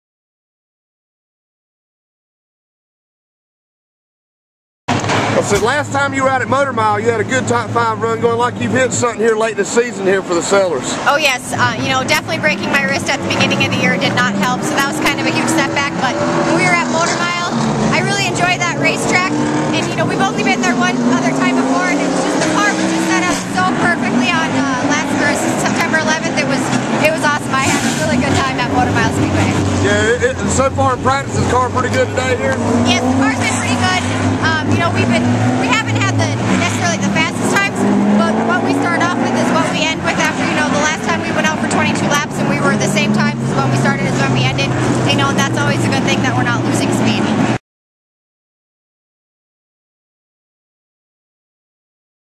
Natalie Sather Interview at South Boston
RACE22 talks with Natalie Sather during practice at South Boston.